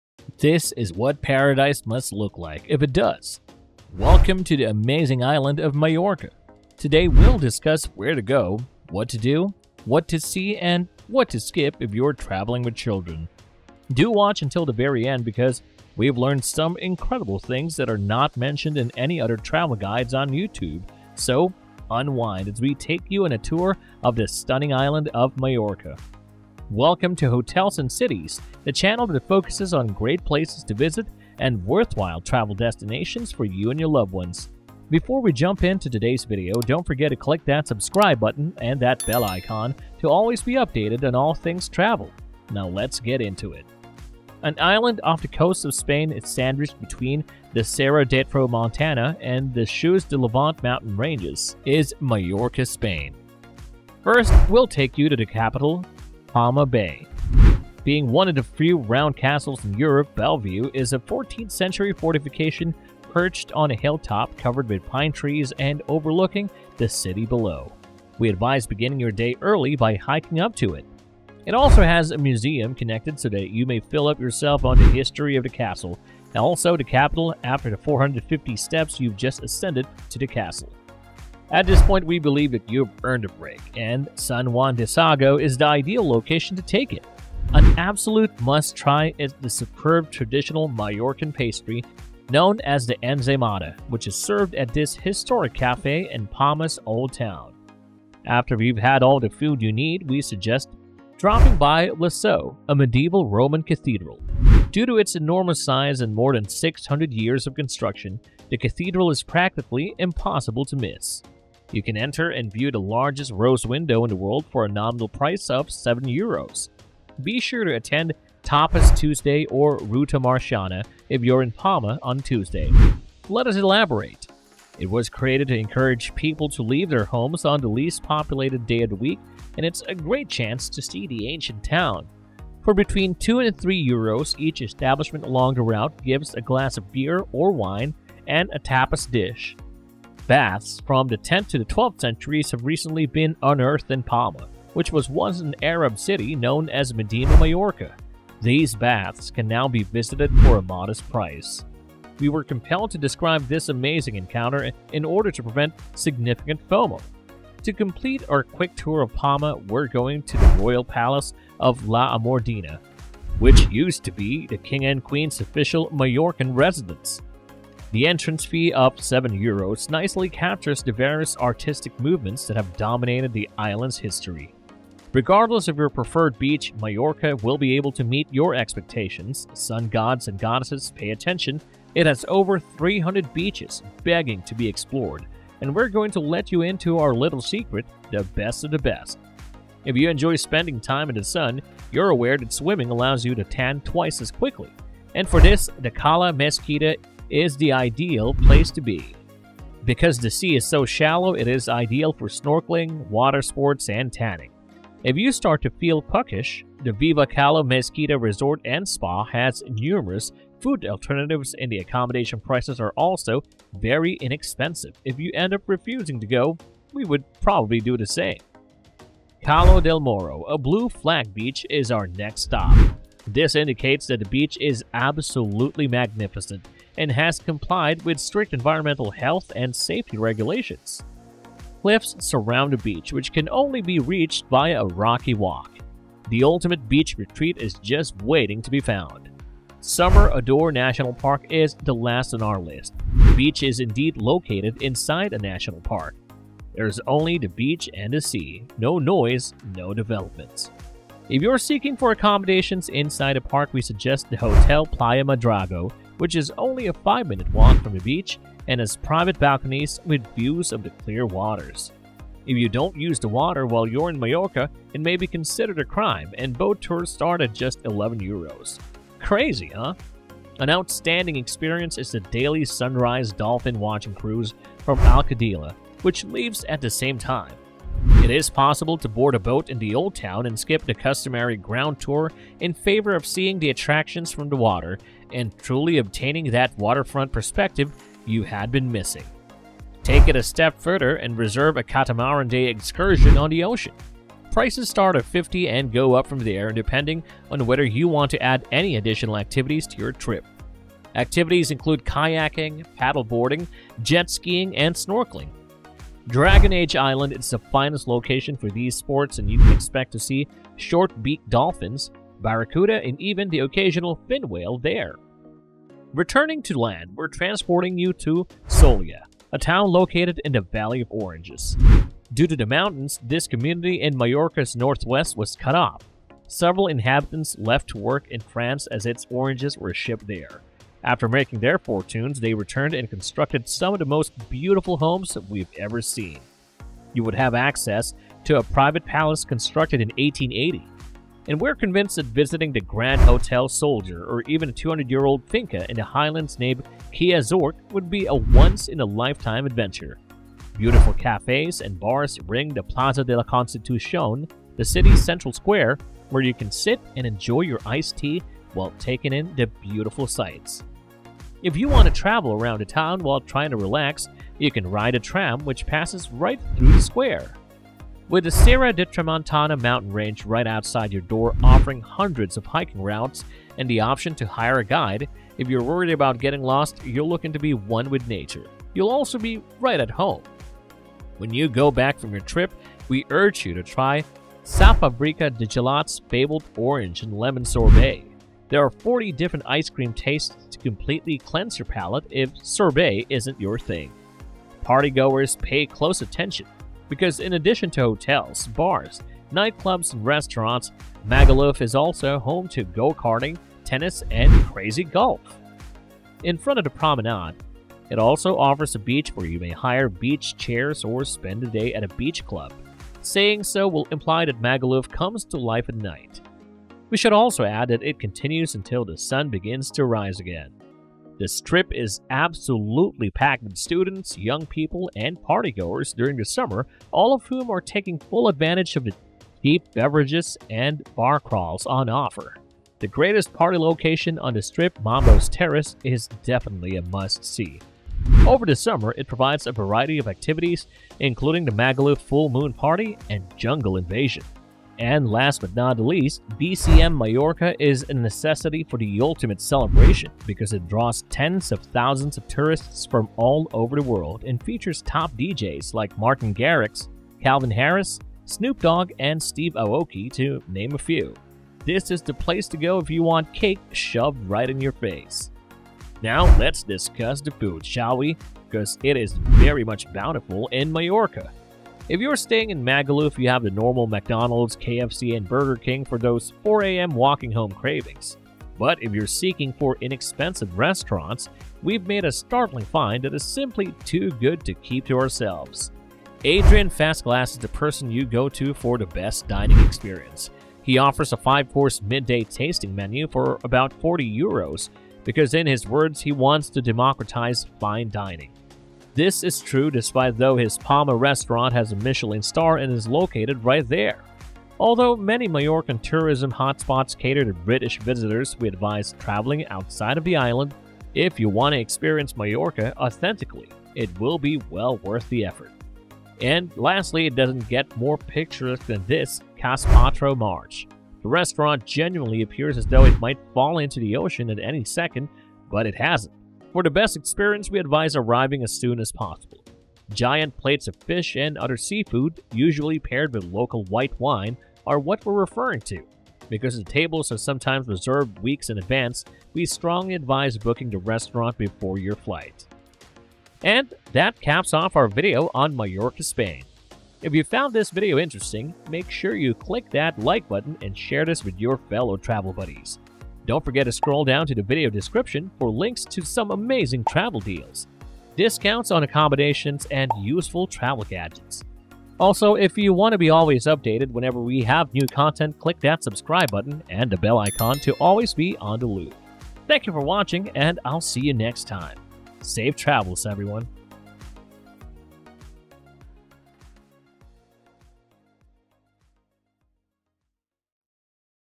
Mallorca Spain Travel Vlog *2023* | Europe guide channel
Welcome to our Mallorca Spain Travel Vlog! In this video, we'll be taking you on a virtual journey to the stunning island of Mallorca, also known as Majorca. Join us as we explore the vibrant capital city of Palma de Mallorca, showcase the island's breathtaking scenery, and share our top recommendations for where to go and what to do during your visit.

As we take you along with us on our travel vlog, we'll provide you with an insider's perspective on some of the best things to do in Mallorca. Whether you're interested in hiking, beach-hopping, or soaking up the local culture, we've got you covered with our comprehensive Mallorca travel guide.

If you're wondering where to stay in Mallorca, we'll share some of our favorite hotels and accommodations on the island. And if you're looking for adventure, be sure to check out our trek travel tour recommendations for an unforgettable experience.

So sit back, relax, and let us show you all that Mallorca Spain has to offer in this Majorca vlog. From stunning beaches to historic landmarks and delicious local cuisine, there's something for everyone on this beautiful island. Don't miss out on the chance to explore one of Spain's most picturesque destinations with us!

If you're planning your next vacation and considering Mallorca as your destination, be sure to watch this travel vlog and get inspired by all the amazing things to do in Mallorca.

🏖 HOTELS & CITIES WEBSITE/BLOG.
Popular videos about traveling